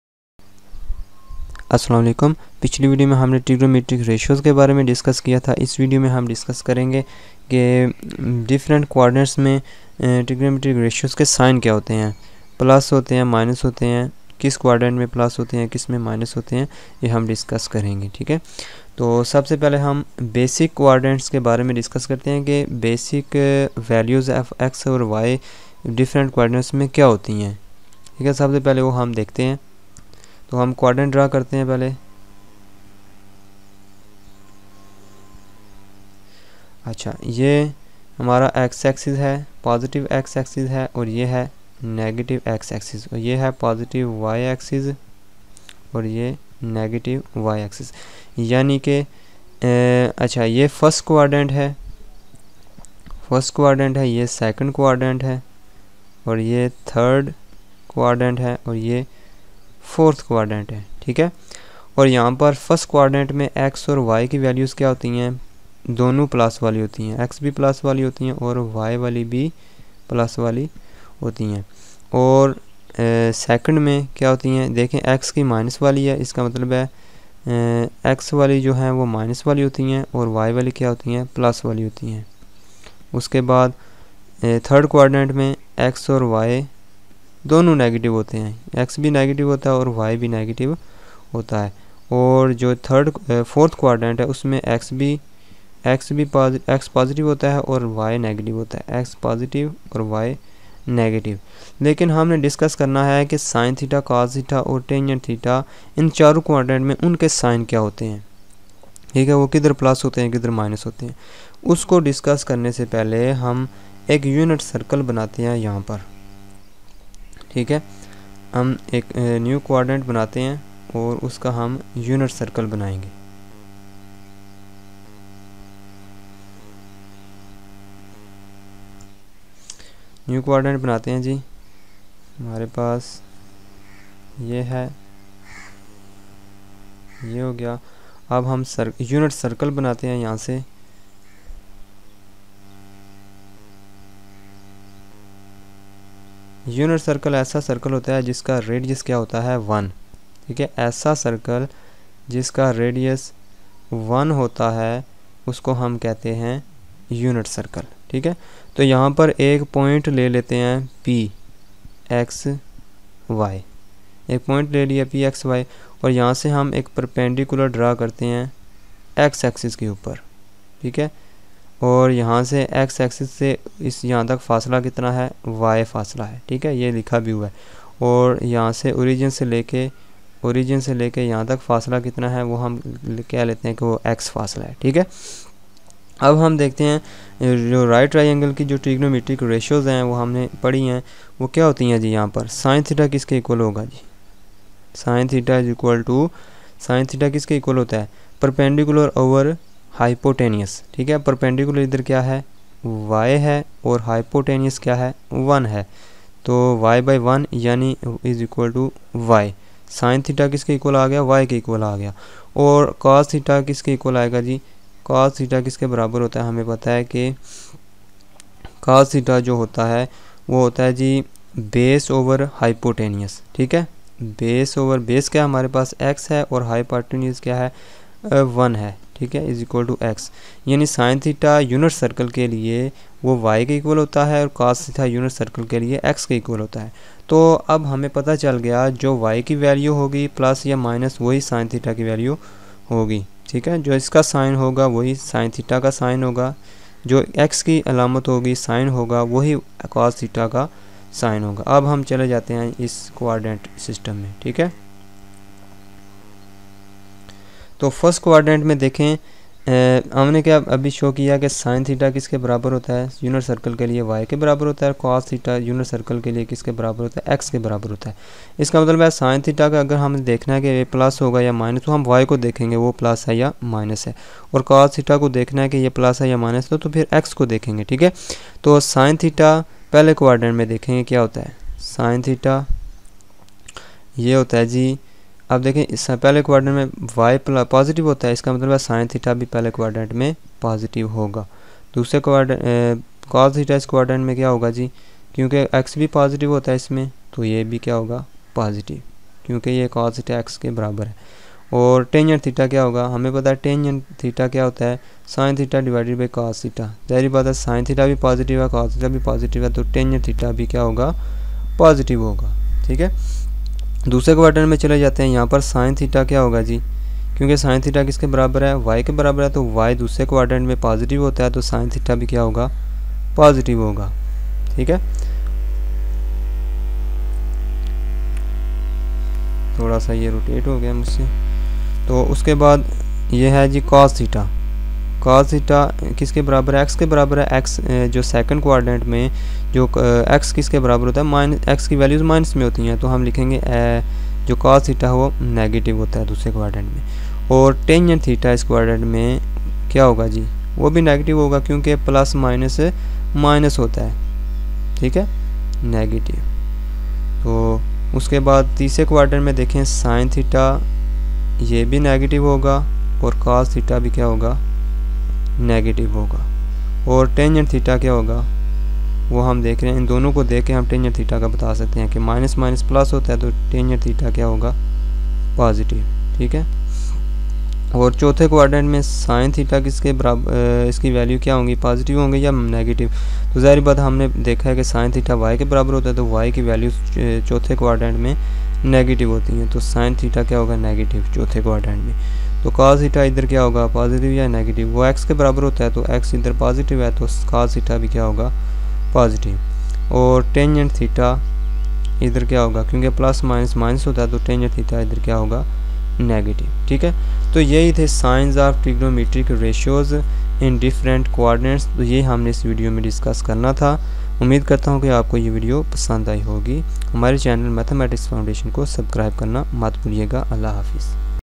Signs of trignometric ratios in Quadrants in Urdu/Hindi 2018 lecture
In this video you will learn the basic concepts of signs of trignometric ratios in quadtrants in urdu hindi.